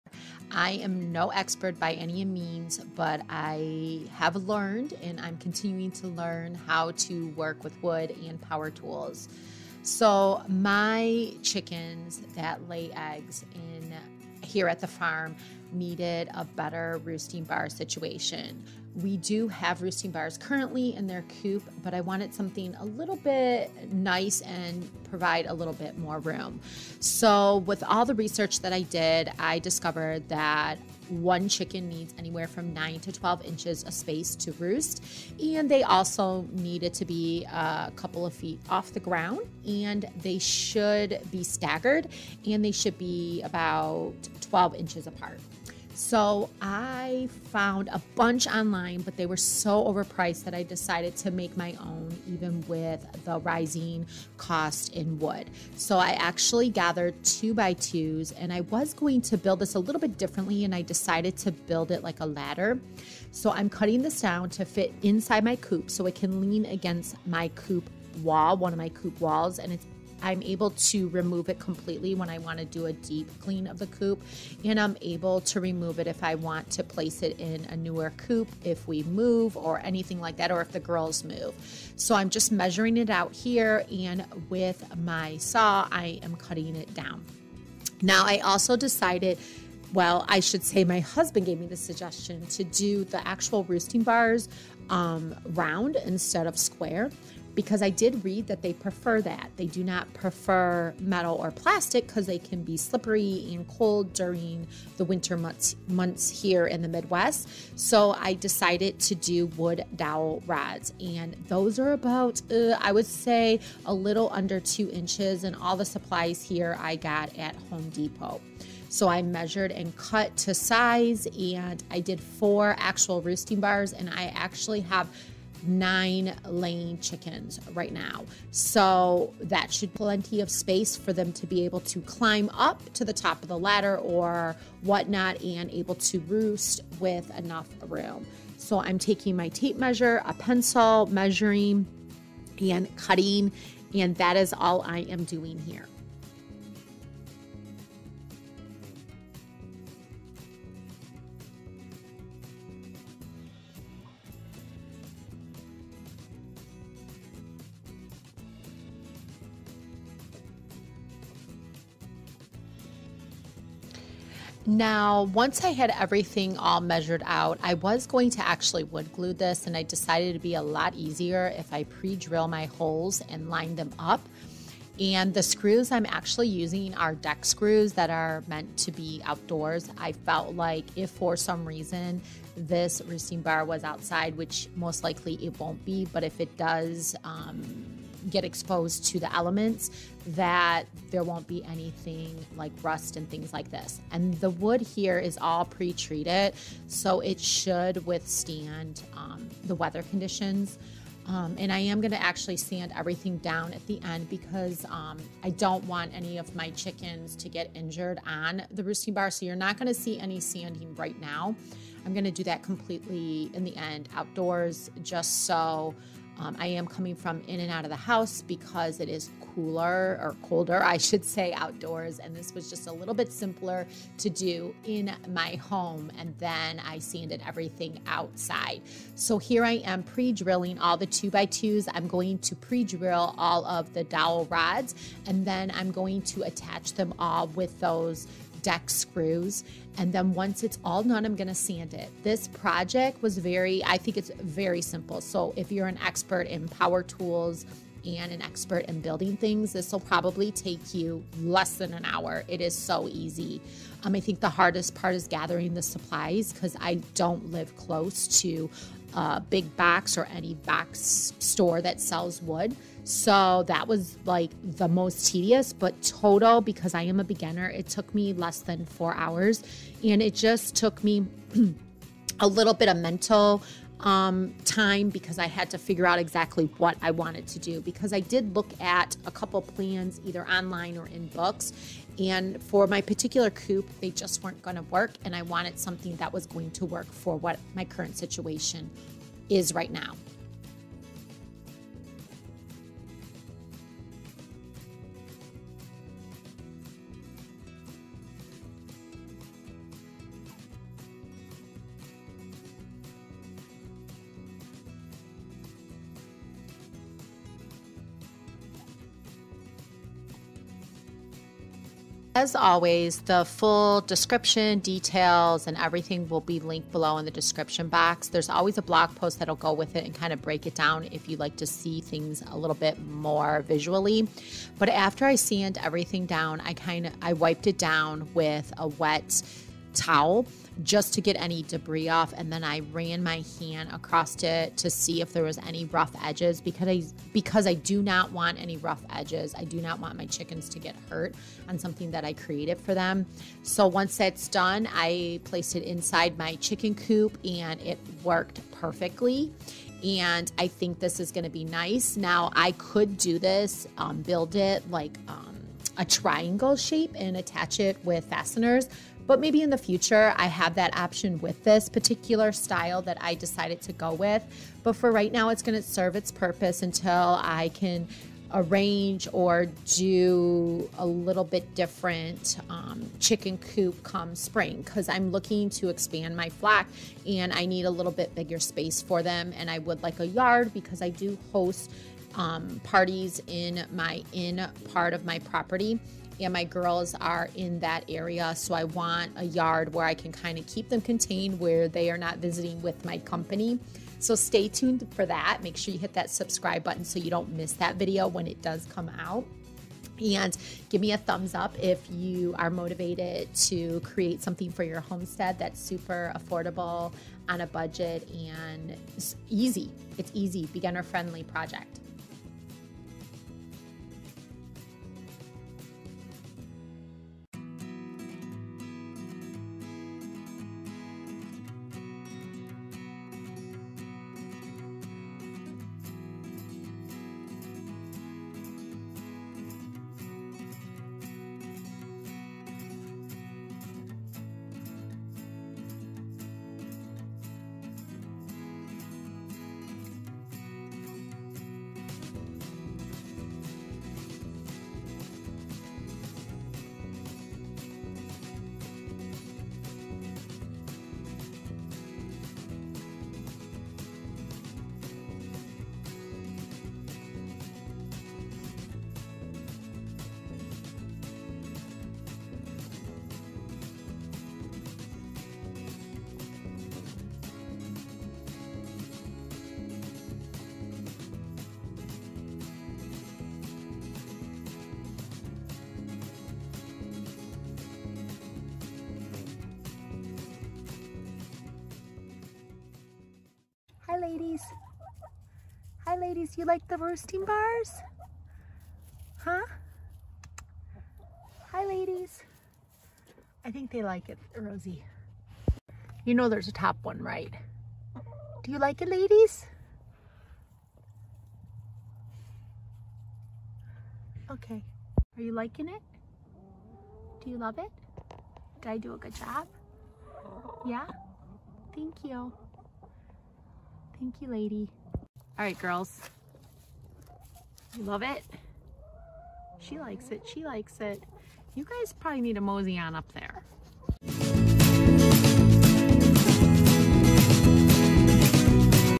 How to Build a Chicken Roosting Bar | Easy Beginner Homestead Project
I notice my chickens were fighting over roosting bars and I solve the problem in four hours.  Let me teach you How to Build a Chicken Roosting Bar for beginners.
Raising chickens provides me with a tremendous amount of joy.  Hens are very interesting to watch, and they also provide my family with fresh, nutrition dense food to bake and cook here on the farm.
These little birds are very easy keep however, they can be very bossy.  Roosting bars in the hen house is a high premium!
Chickens do not like to sleep in their nesting boxes or at ground level.  After owning my ladies for a year, they love to sleep high, and they fight for the primo spot.

Details on building this beginner roosting bar head to the blog for details~

FAVORITE PRODUCTS / ITEMS